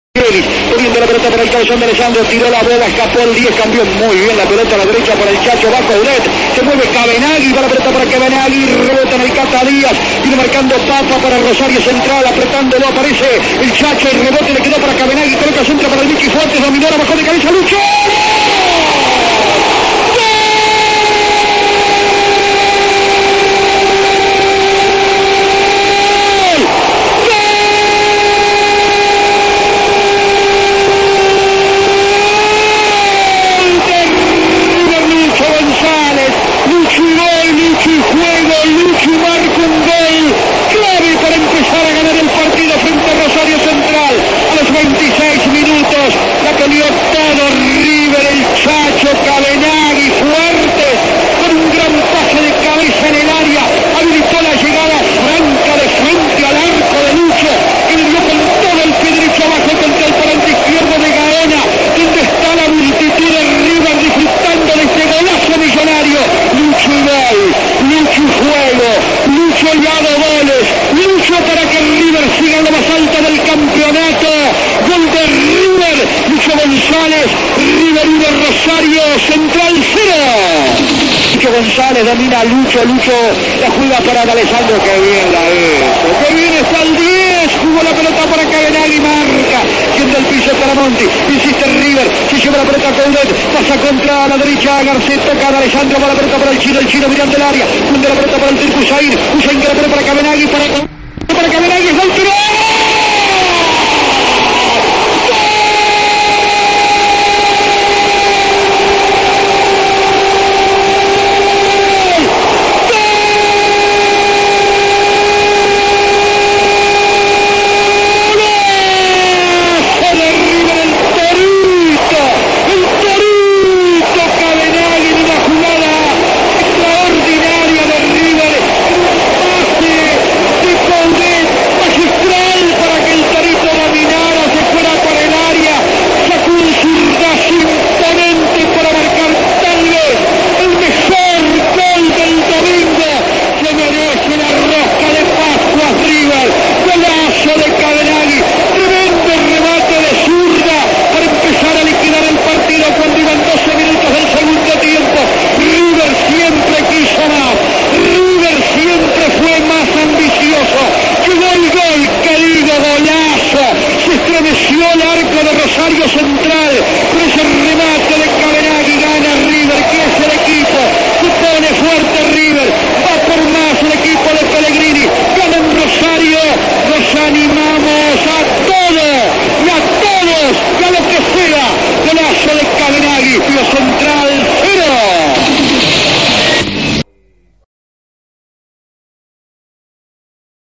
River 2 Central 0 Clausura 2003 (Costa Febre)
Los goles de Luis Gonzalez y Cavenaghi en ARROYItO CON RELATO DE Atilio Costa Febre